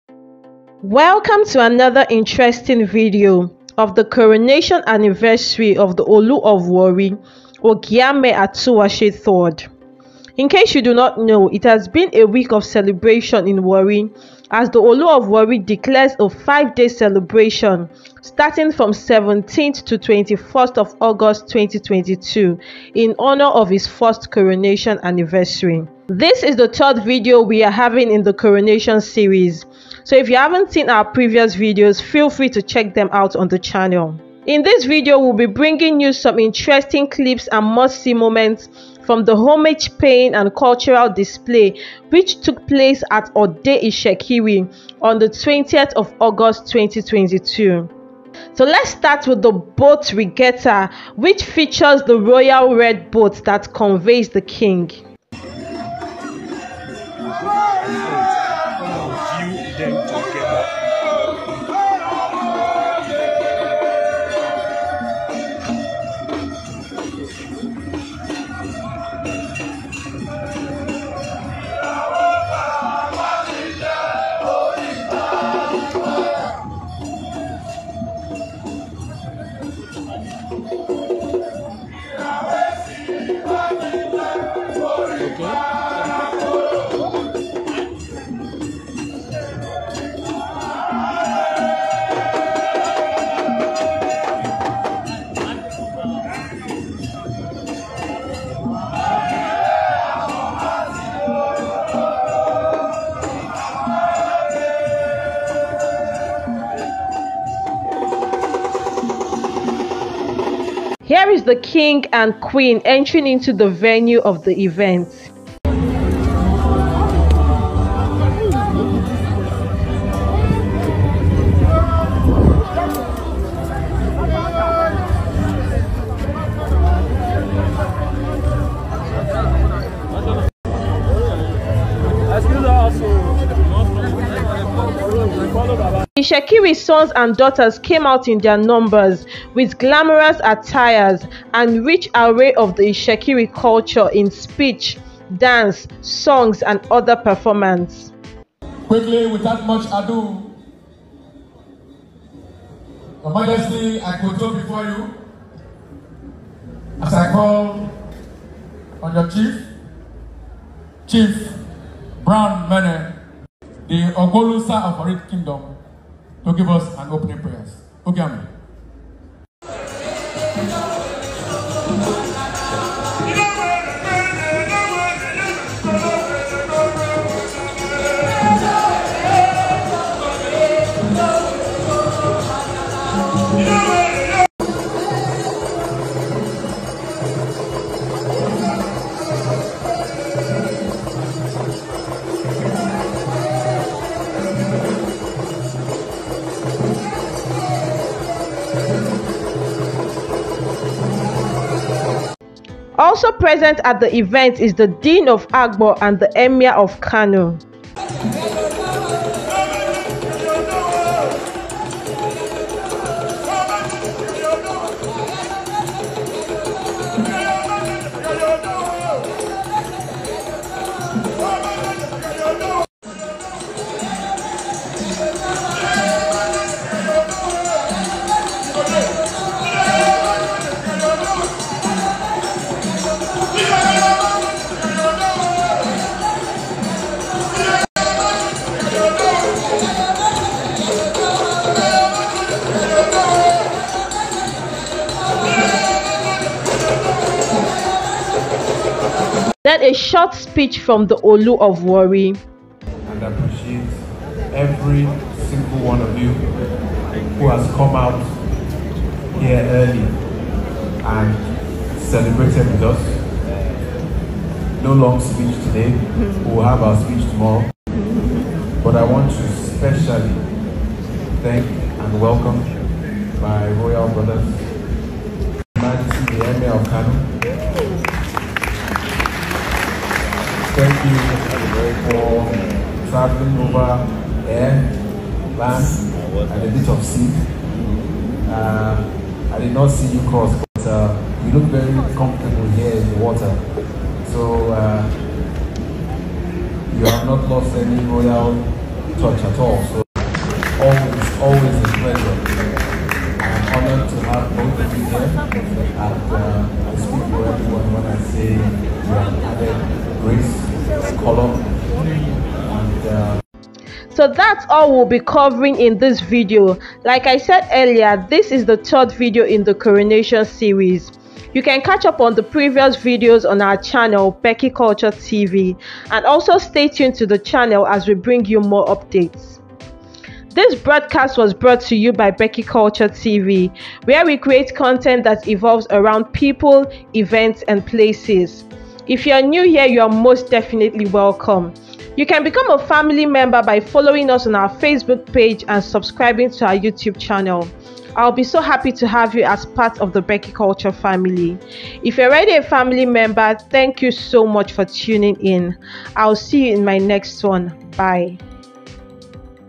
Olu of warri first coronation anniversary. Cultural display of the rich itsekiri traditions
If you have any video request or questions about Lagos Nigeria, please drop it in the comments section or send a DM on Instagram